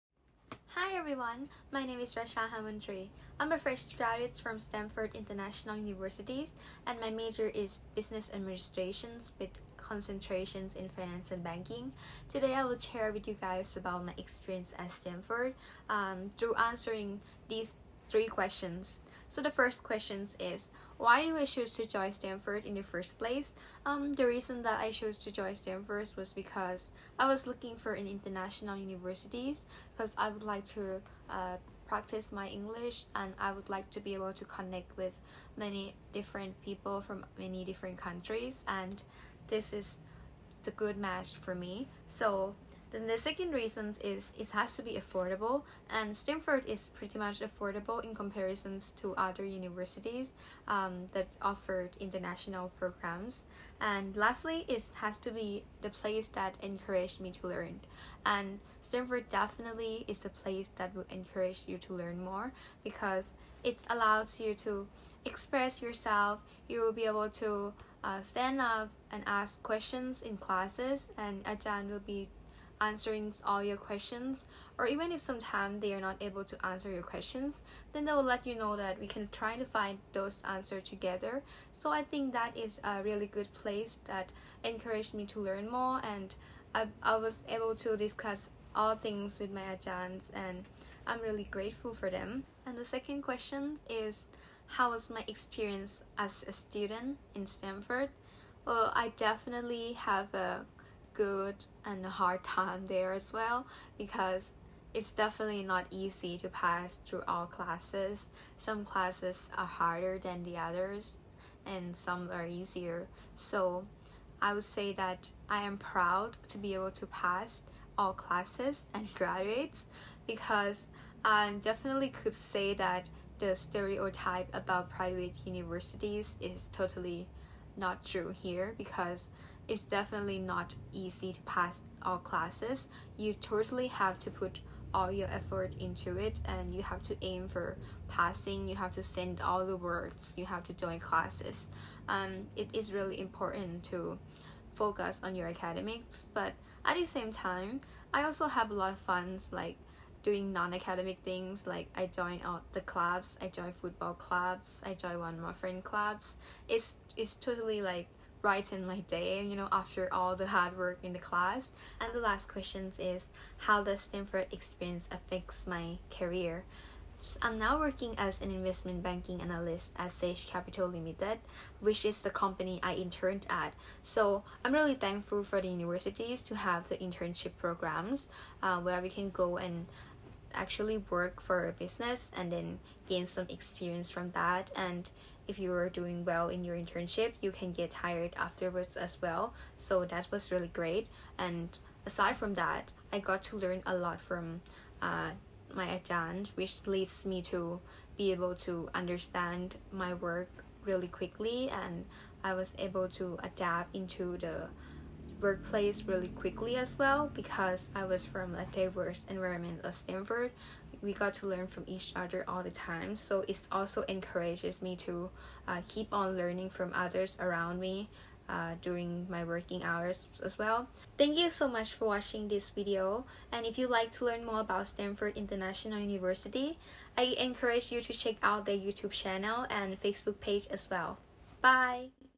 Student Testimonials: Racha Hamontree – Finance & Banking
Listen to what current students and alumni have to say about their Stamford experience